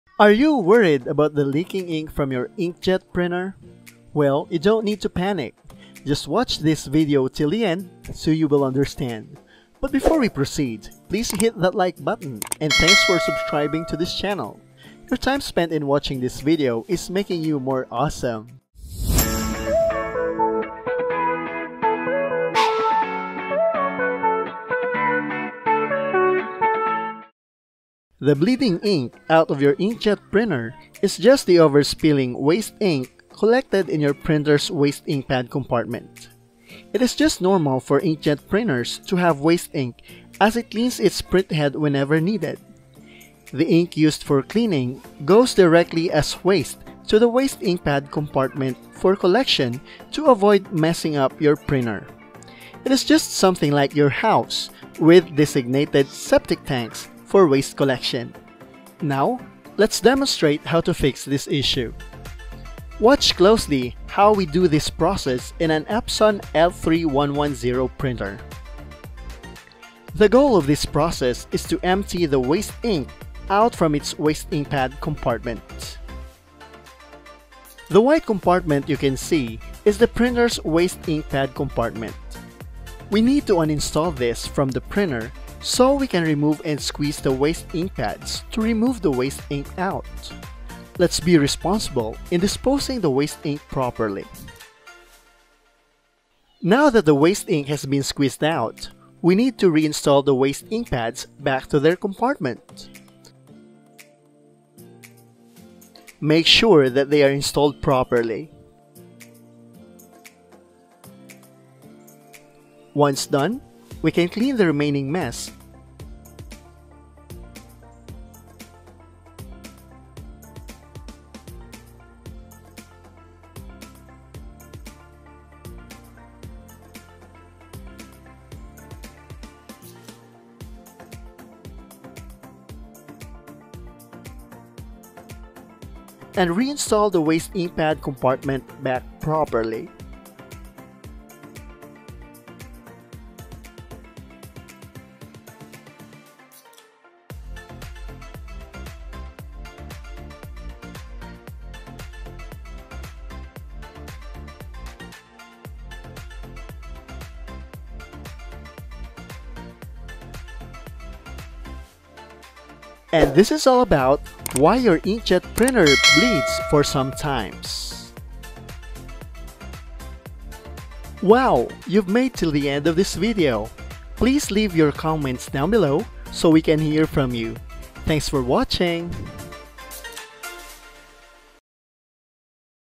Does Your Printer Bleed? Here's Why and How to Fix It!
This video explains why ink from inkjet printers leak. The step by step procedure has been illustrated for guidance. The same principle applies to other inkjet printers. In this video, however, Epson L3110 has been used for demonstration purposes.